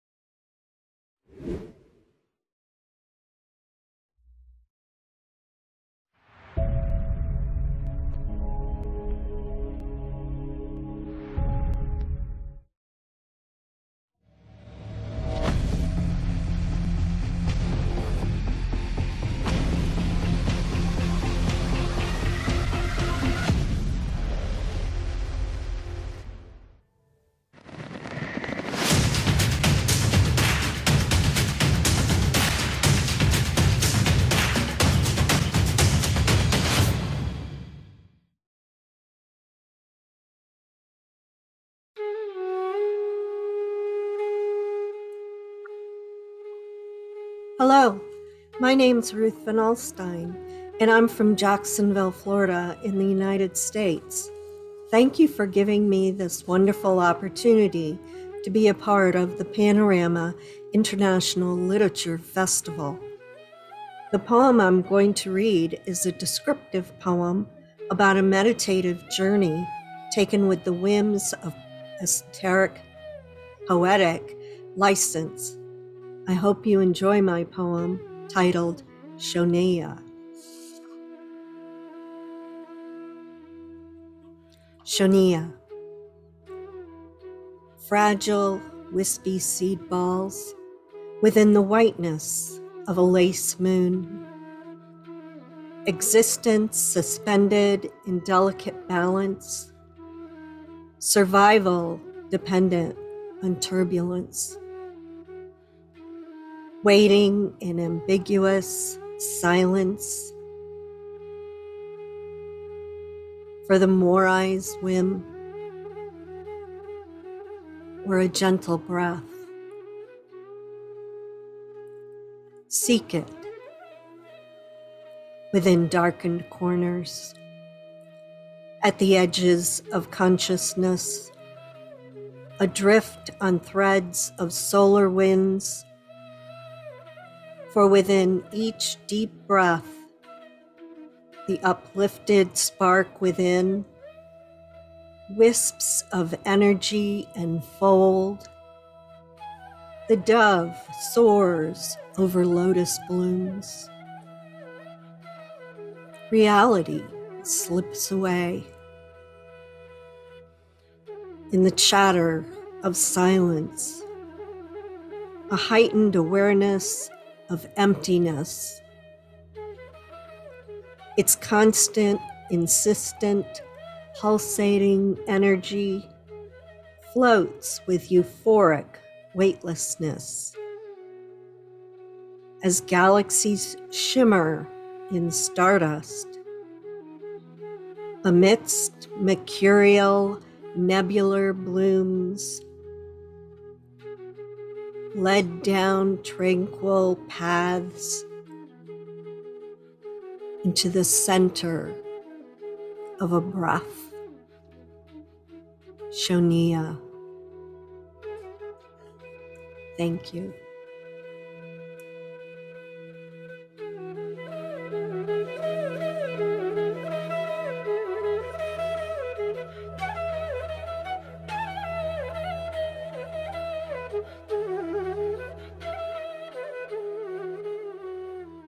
Panorama International Literature Festival 2022: Ruth Van Alstine, New York, USA
Panorama International Literature Festival 2022

Theme: Shoonya: The Celestial Void
From 1-30 January 2022 Worldwide
Presentation: Ruth Van Alstine, New York, USA